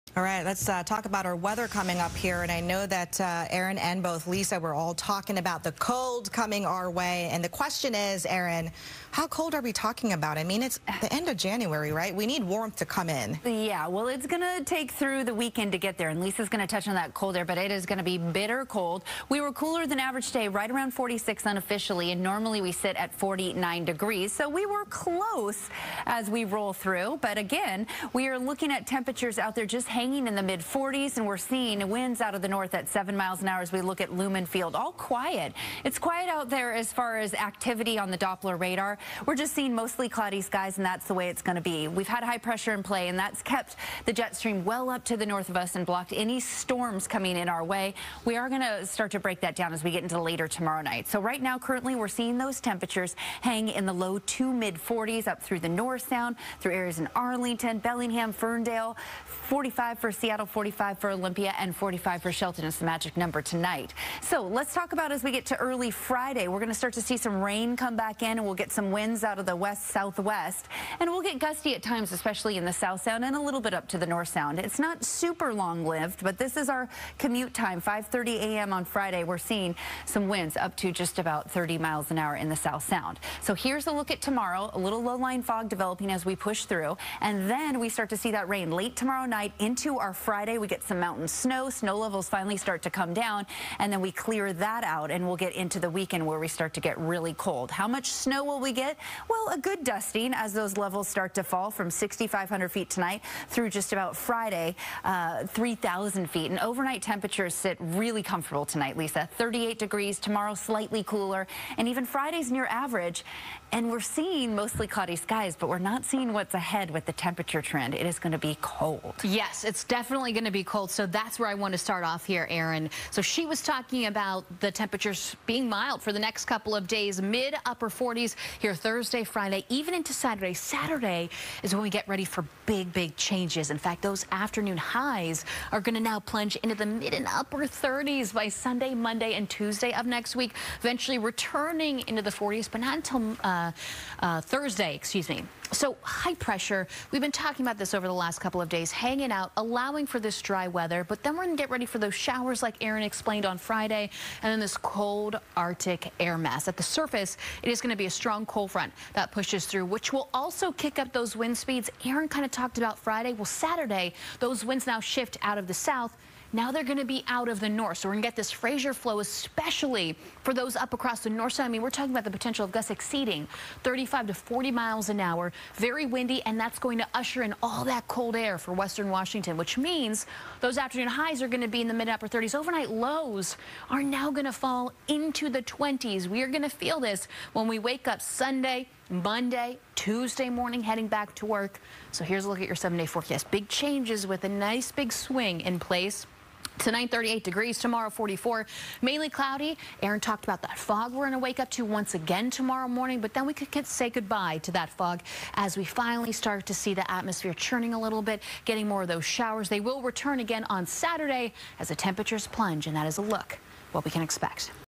Breezy weekend and then a COLD week ahead | FOX 13 Seattle
FOX 13 Chief Meteorologist Lisa Villegas and Forecaster Erin Mayovsky have the details.